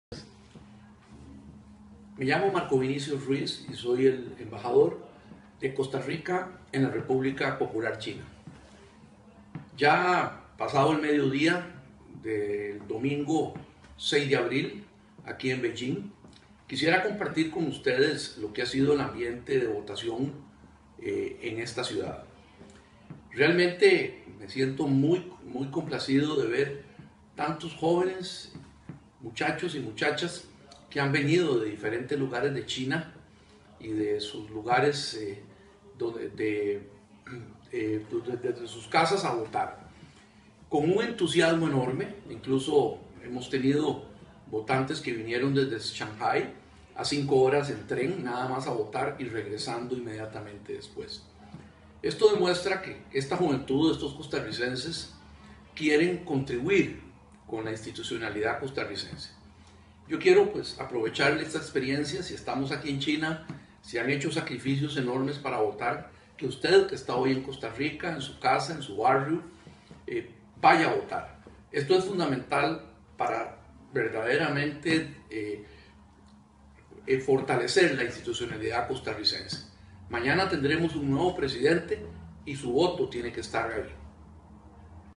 Mensaje Marco Vinicio Ruiz
Acá les dejamos este video del embajador de Costa Rica en China, invitando a todos los costarricenses para que salgamos a votar.